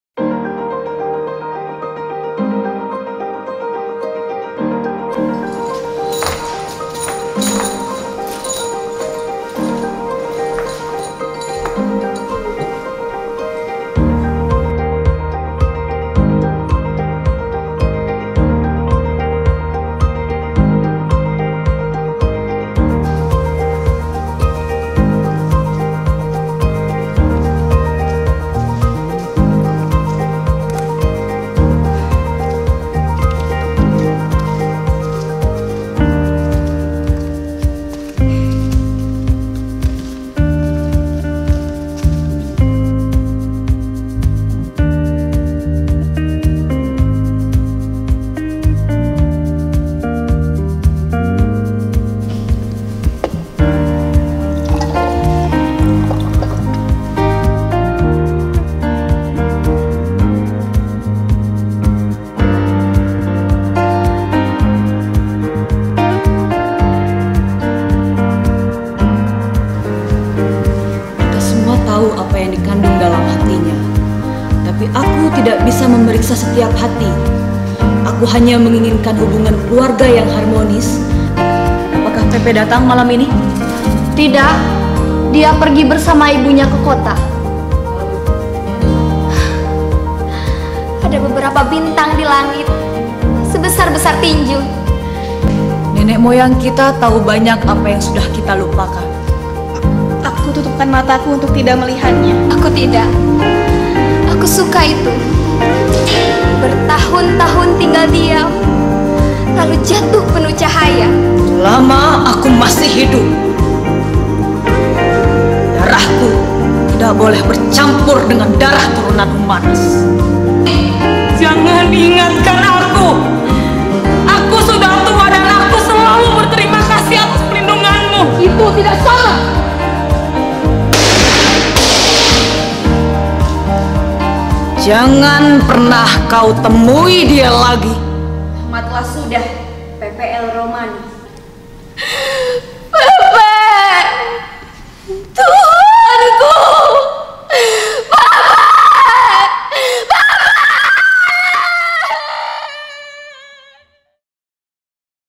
proses garap "RUMAH BERNARDA ALBA"
naskah terjemahan RAMADHAN KH, yang di garap oleh mahasiswa jurusan Teater untuk TUGAS AKHIR STUDI S1, yang dilaksanakan di gedung kesenian sunan ambu ISBI BANDUNG